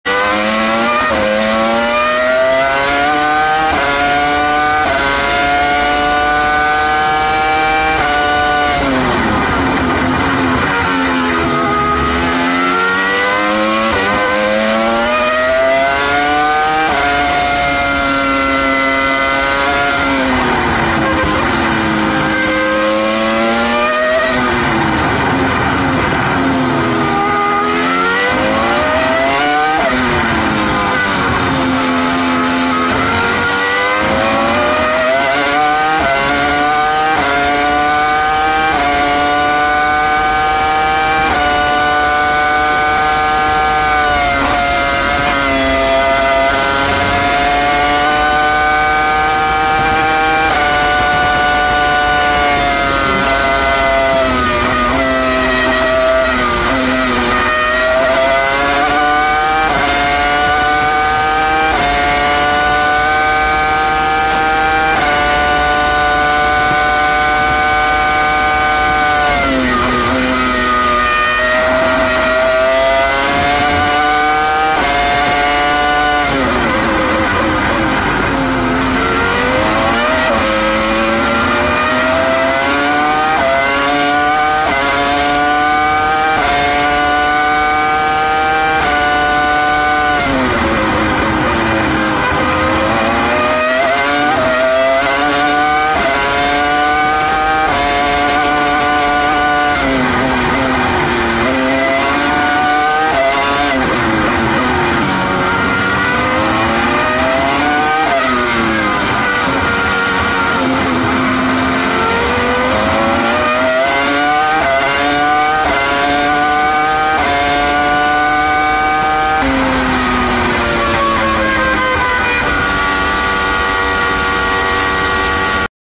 F1 Classic Sounds Juan Pablo Montoya Williams BMW V10 Onboard Great Britain 2004
Experience Juan Pablo Montoya’s onboard lap in the Williams BMW V10 during the 2004 British Grand Prix. Witness precision driving and the roaring V10 engine in this thrilling moment from F1 history.